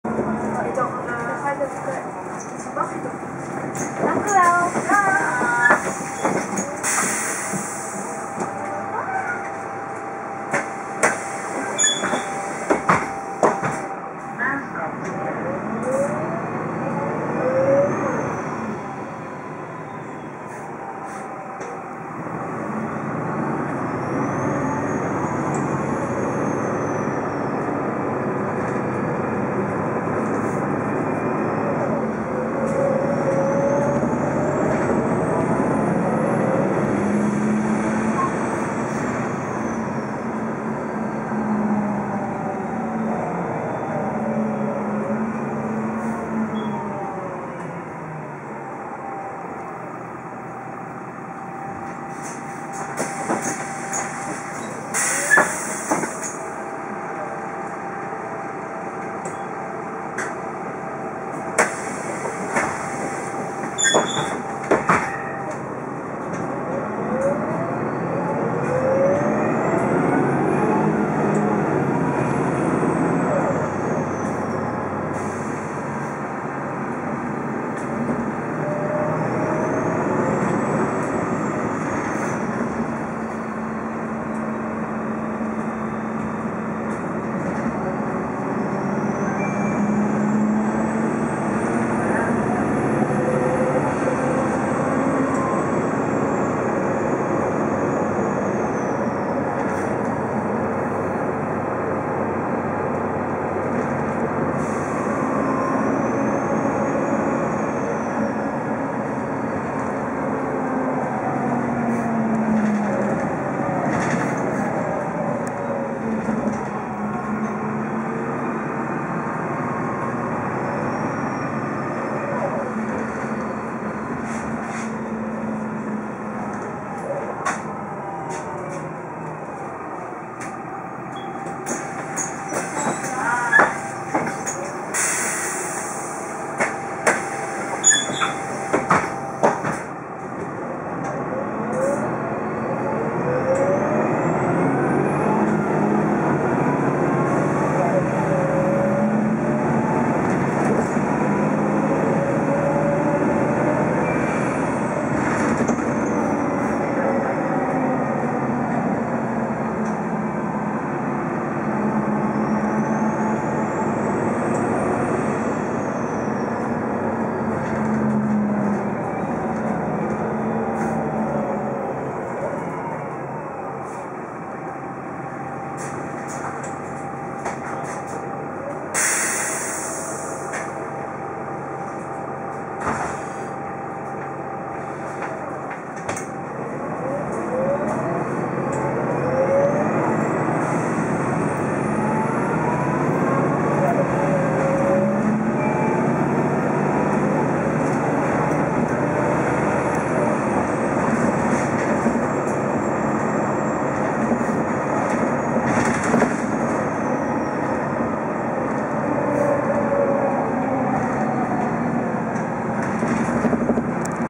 Arriva 728 | Mercedes-Benz O530 Citaro (ZF Kickdown)
Hierbij een ritopname in de Arriva 728 op 19 juli 2011, waarbij deze bus met uitzondering eens met de kickdown rijdt, dat gebeurt niet vaak in Dordrecht. Het betreft een Mercedes-Benz O530, beter bekend als Citaro.

Kickdowns:
01:05
02:20
03:06